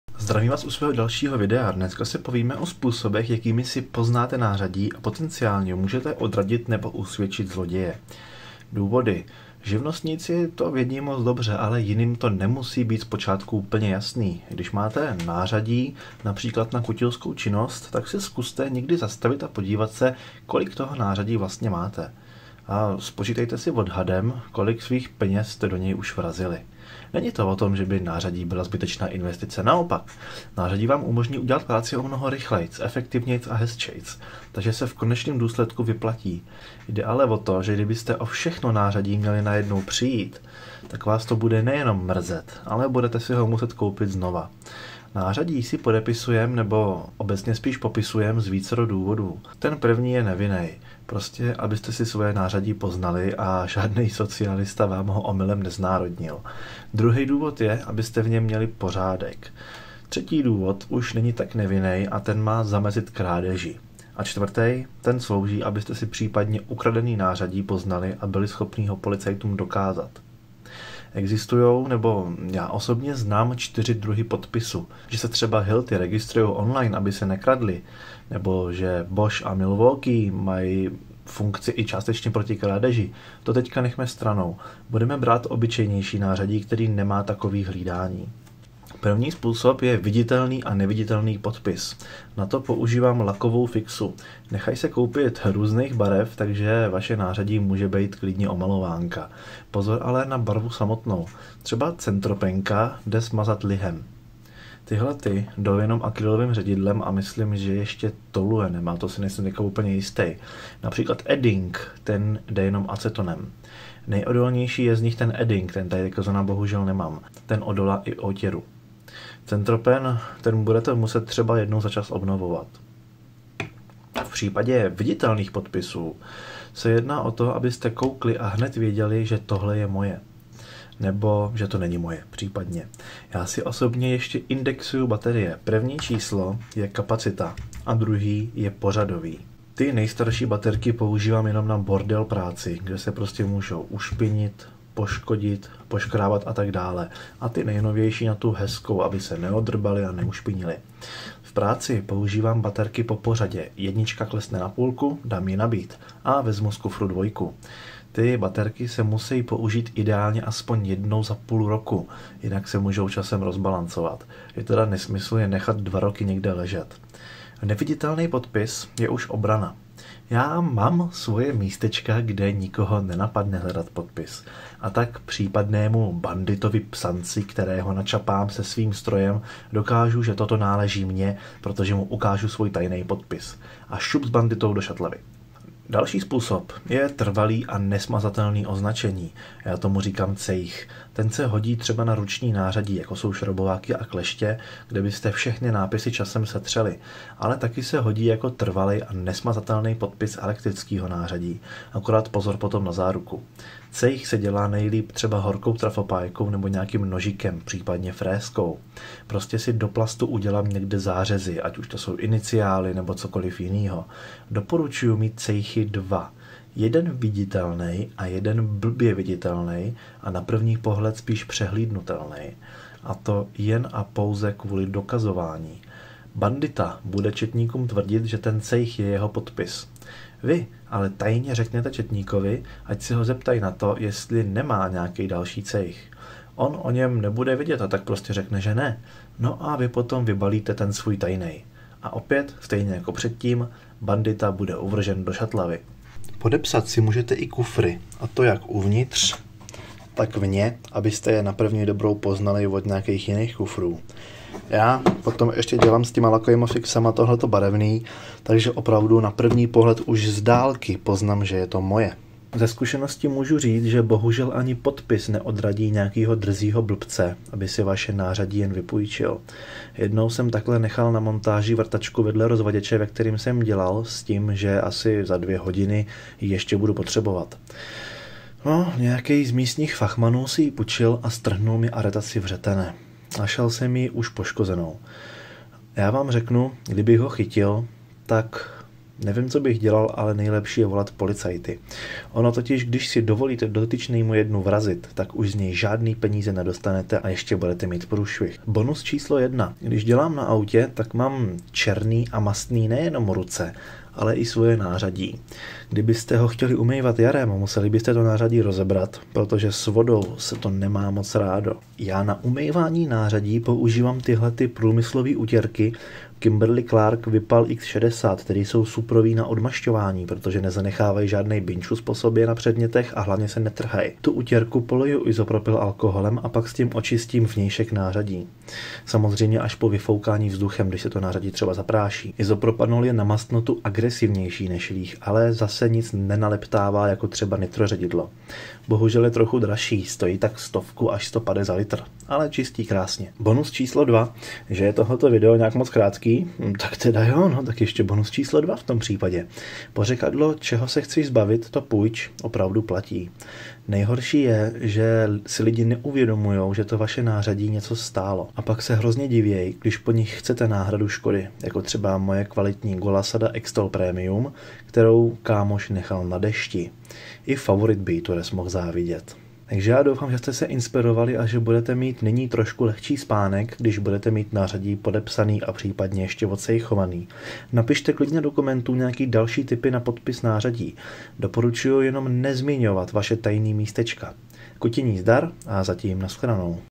Jak nepřijít o nářadí
V tomto videu si řekneme poněkud smutné důvody, proč je nutné si nářadí podepisovat a cejchovat. Jako bonus si povíme jak očistit mastné nářadí aniž by se poškodilo.